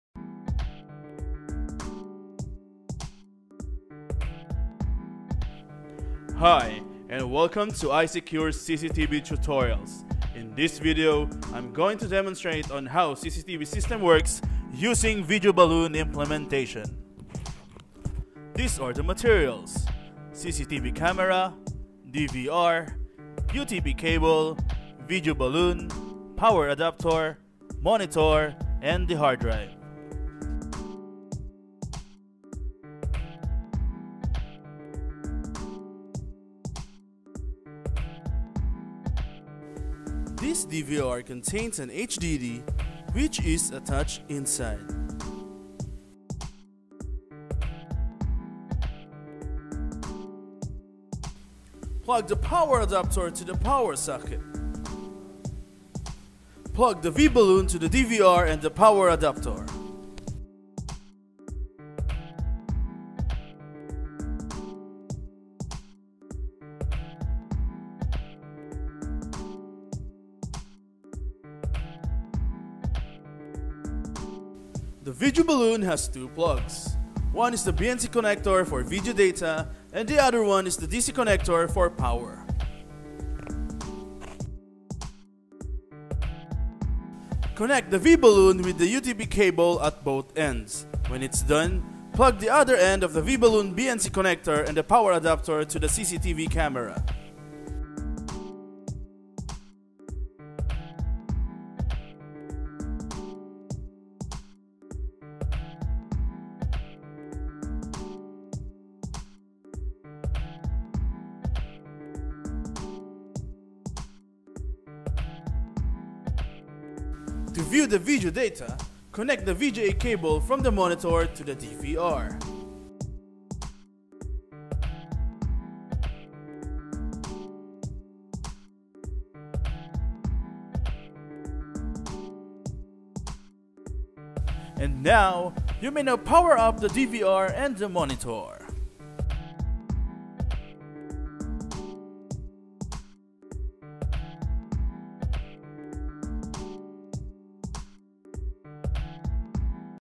iSecure CCTV Video Balun Implementation Tutorials
This video tutorial shows the components needed and how to install cctv system using UTP cable (cat5e) with balun converters.